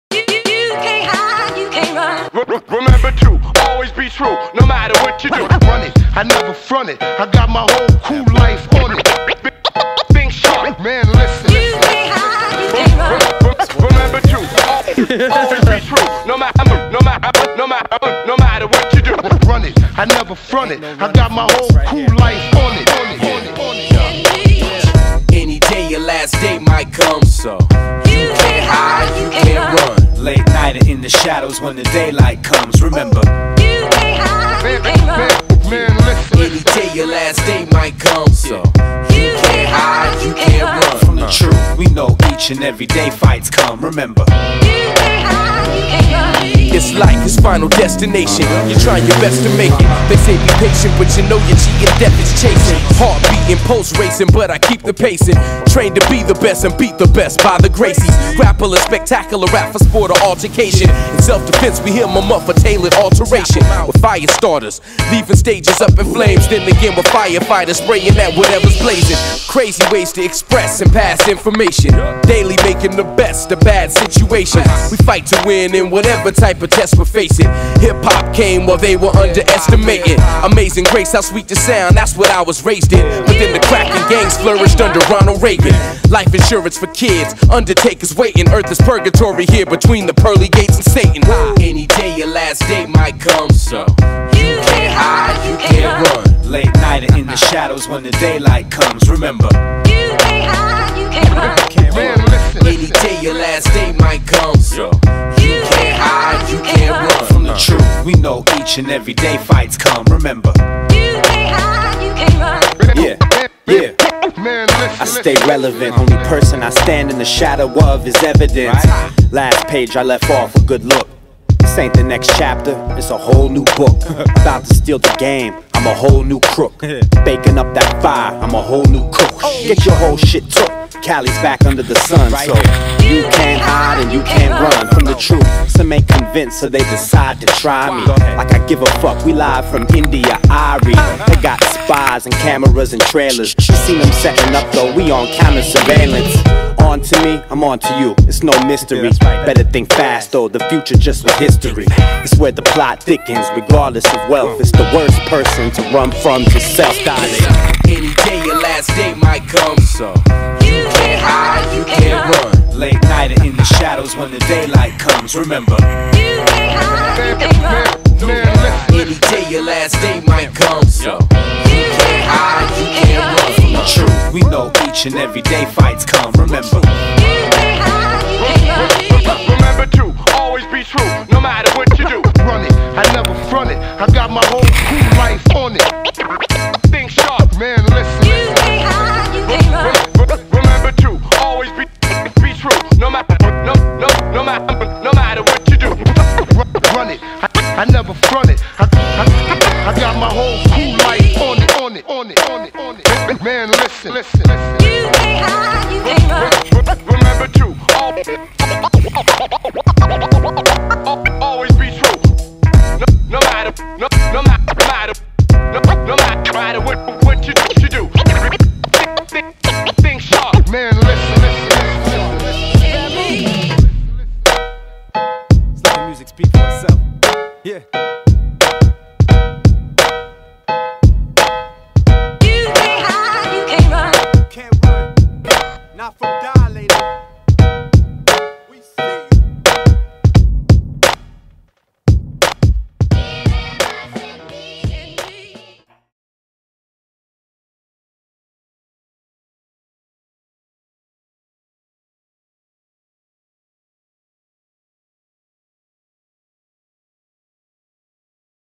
Wakeboarding: You Cant HIde You Cant Run ft. Hunter Schleper
You Cant Hide You Cant Run From 2010. a lil edit i threw together of 2010. rider hunter schleper